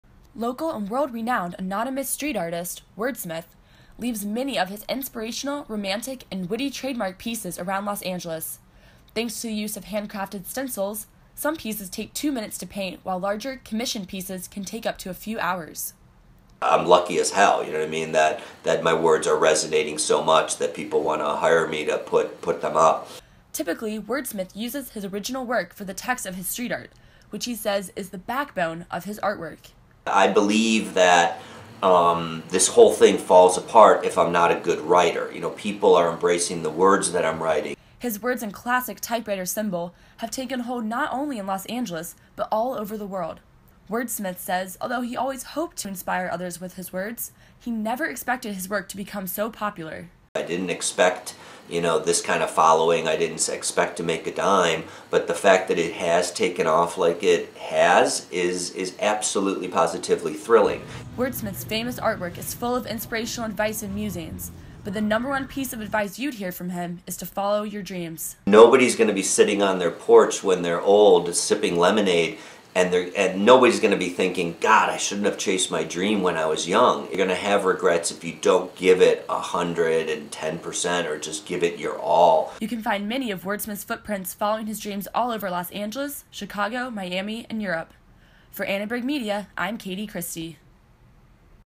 207 FINAL broadcast piece- WRDSMTH
WRDSMTH profile/interview created for USC Annenberg School of Journalism's JOUR-207 Reporting and Writing I final project with Prof. Lisa Pecot-Hebert.